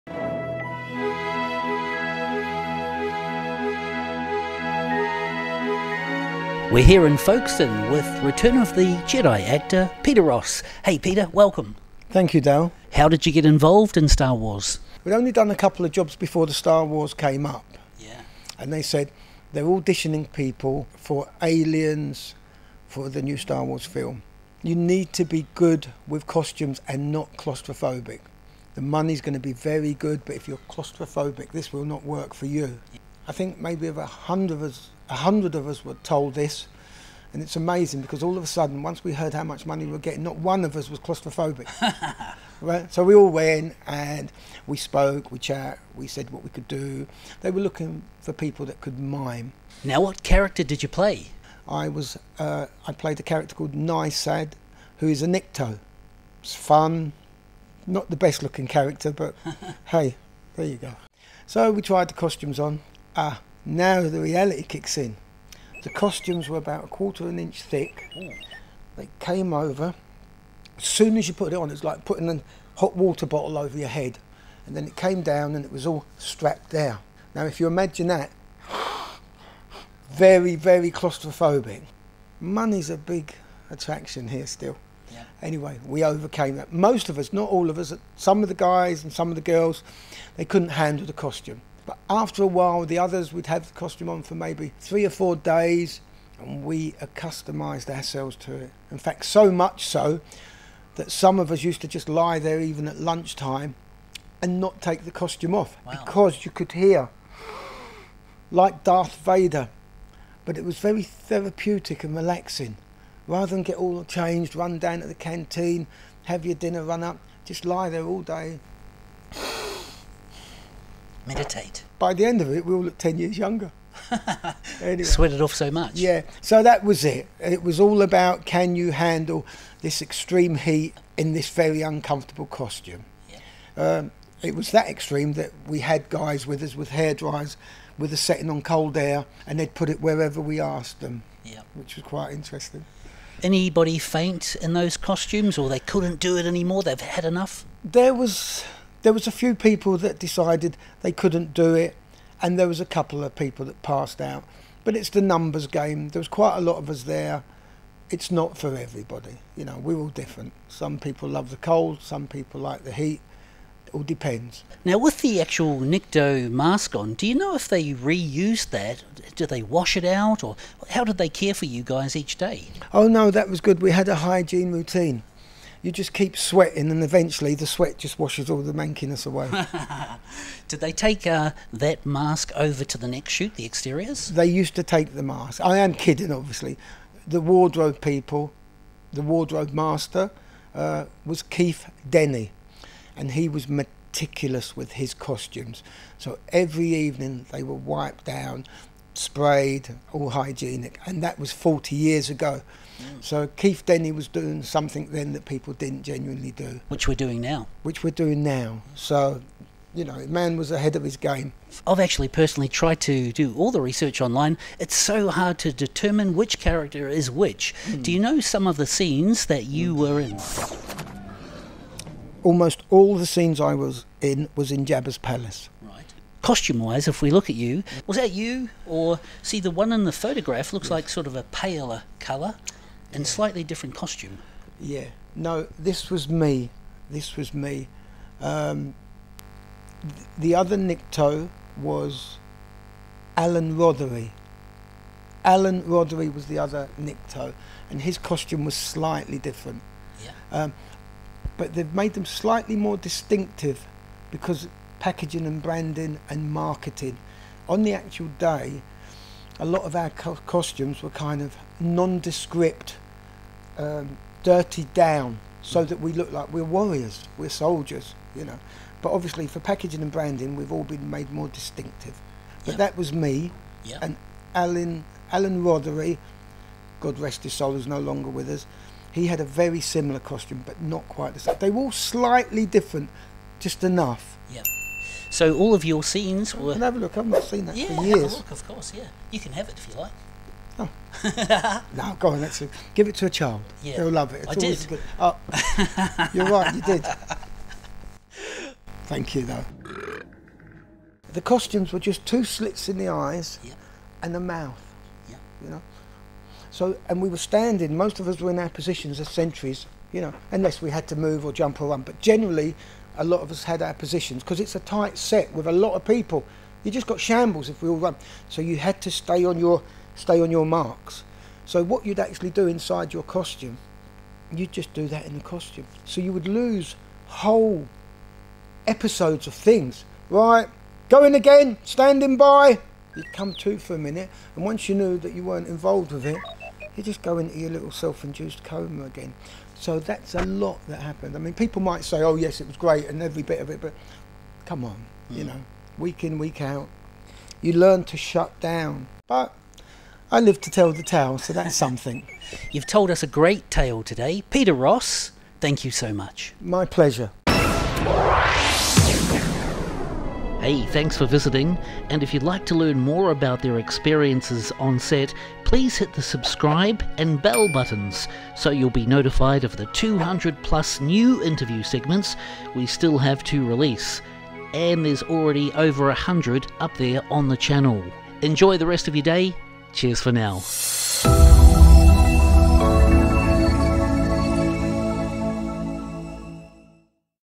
Nikto Unmasked - PETER ROSS Interview (Part 1) - Star Wars 100 Interviews
In this chat, (Part 1 of 3) DJ unmasks his second NIKTO character interviewed. Meet PETER ROSS who played the interior scenes that were filmed in the U.K. for Return of the Jedi. Stay for the entire interview, as DJ showcases very rare footage and 3 rarely seen photos from Peter's own, personal collection.
He played other characters too, which we will save for another segment.
For Nikto fans, after viewing this one, you can hear from one of the exterior shoot stuntmen who played 2 Nikto's - that was Paul Weston. We'll link to that at the end.
Enjoy this fun, fact finder and check out over a hundred more around the channel.